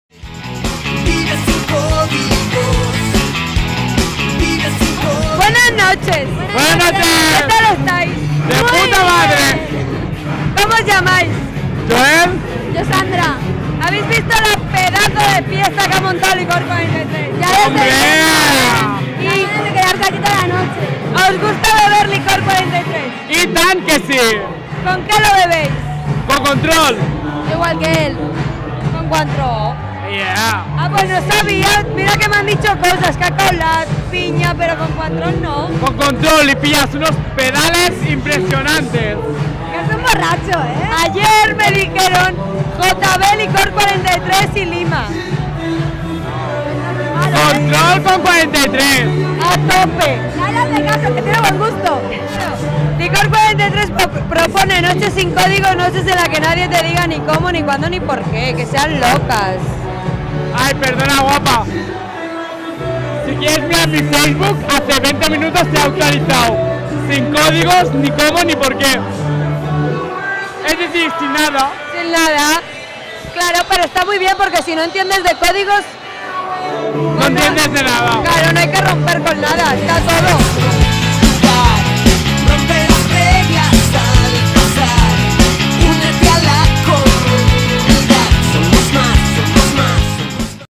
Entrevista Joel y Sandra en Antic Lleida Comunidad Sin Códigos
Entrevista realizada en la fiesta de Licor 43 en Antic Lleida para la Comunidad Sin Códigos.